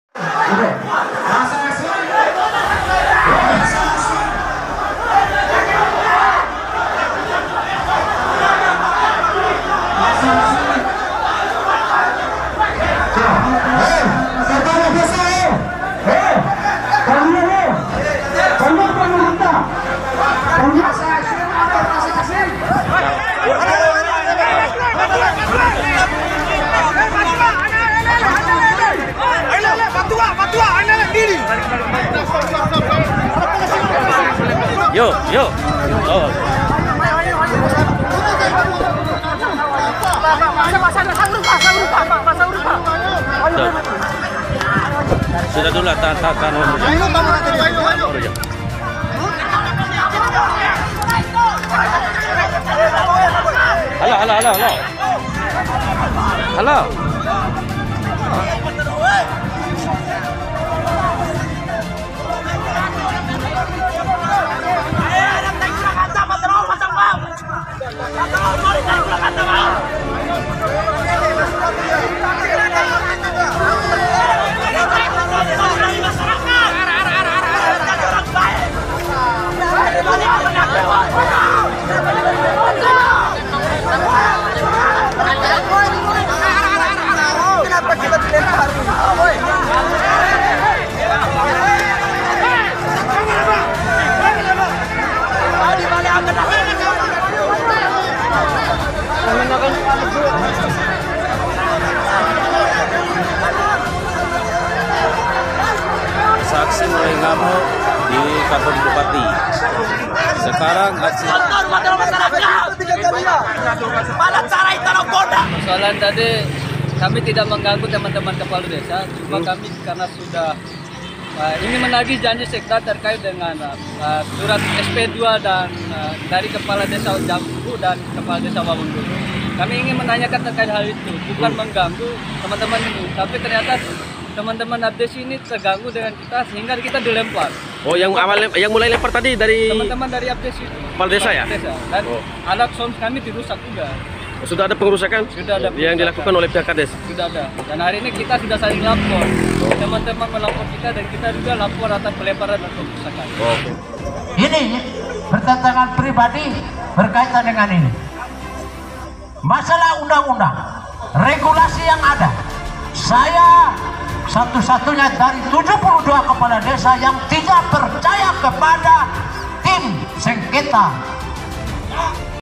DEMO RICUH KADES
DEMORICUH# KADES# DETAK NEWS#

AKSI KEPALA DESA SE KABUPATEN DOMPU, DI DEPAN KANTOR BUPATI DOMPU BERLANGSUNG RICUH, KERICUHAN TERJADI LANTARAN SEJUMLAH AKTIVIS MELAKUKAN ORASI DAN DINILAI MENGGANGGU KEGIATAN KADES TERSEBUT AKIBAT AKSI SALING LEMPAR DAN PUKUL TIDAK TERHINDAR AKSI SALING LEMPAR ANTARA FORUM KEPALA DESA REDA SETELAH DIPISAHKAN OLEH APARAT KEPOLISIAN POLRES DOMPU